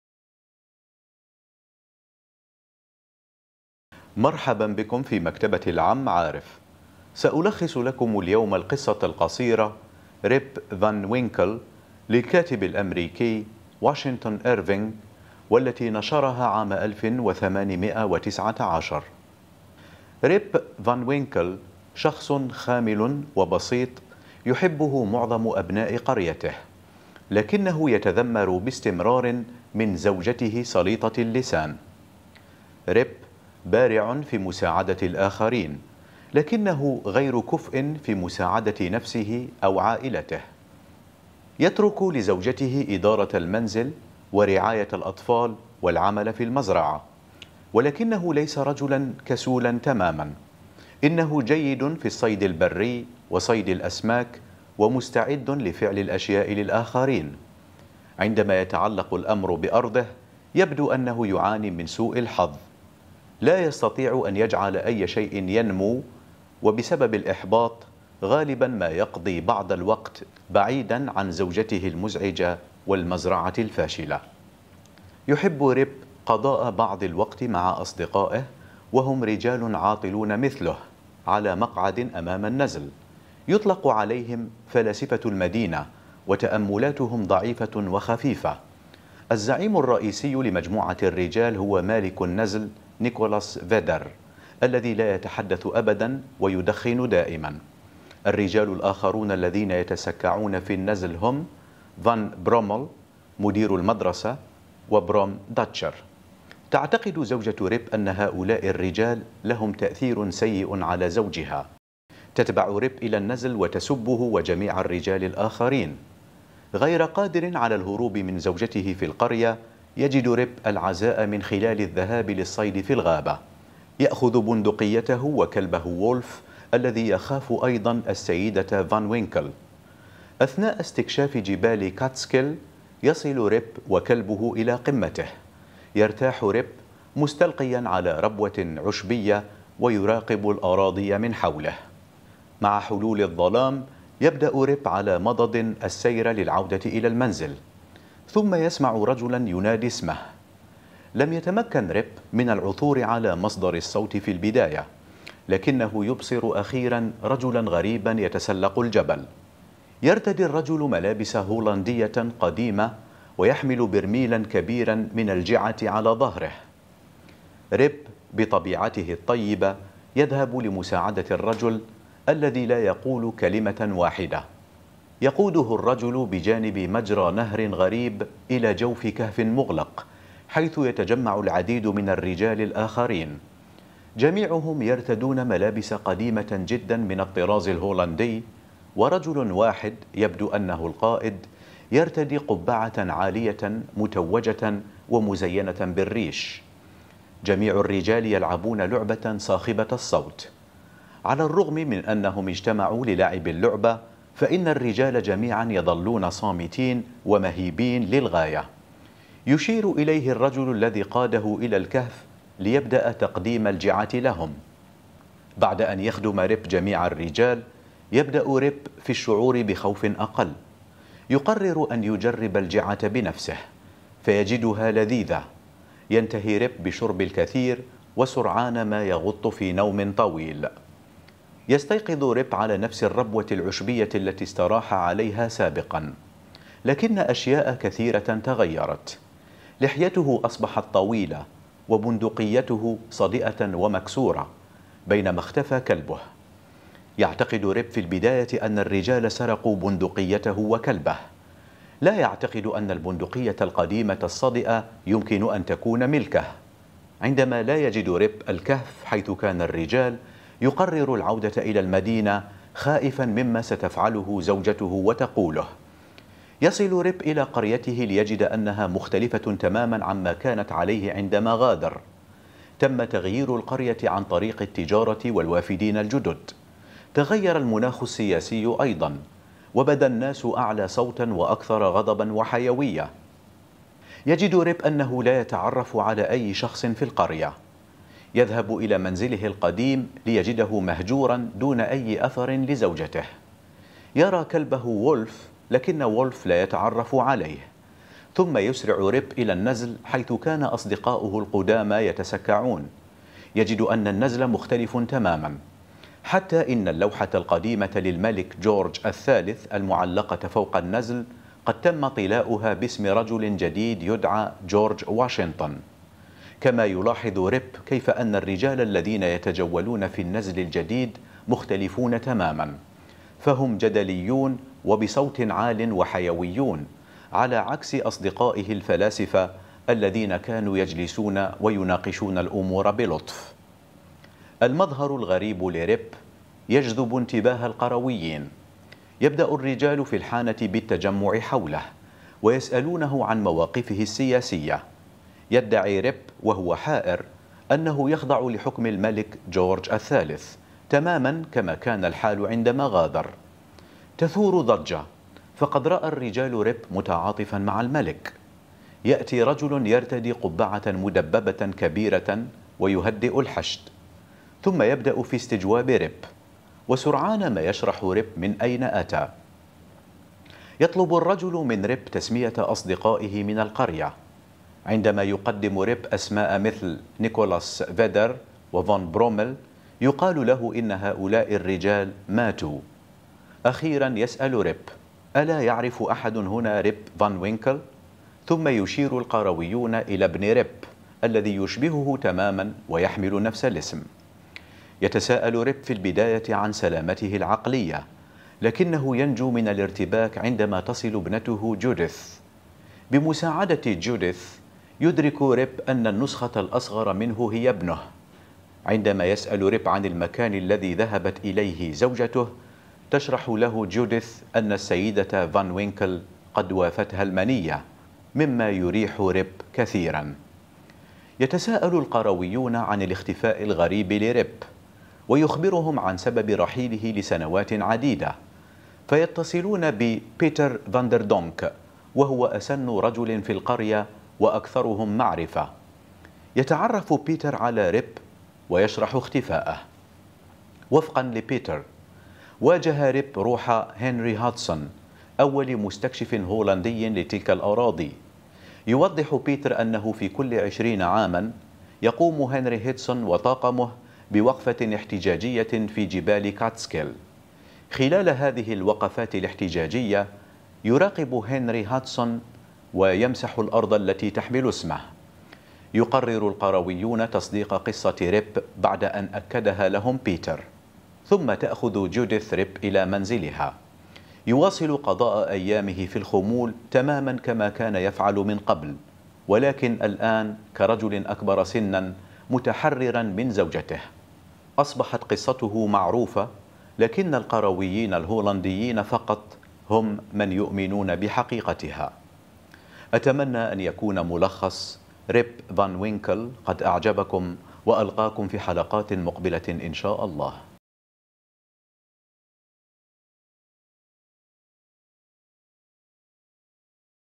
ملخص القصة القصيرة "ريب فان وينكل" للكاتب الأمريكي واشنطن إيرفينغ
هذه القناة ترفيهية وتغوص في عالم الكتب لتقدم للمشاهدين خلاصة لأشهر الكتب في الكثير من المجالات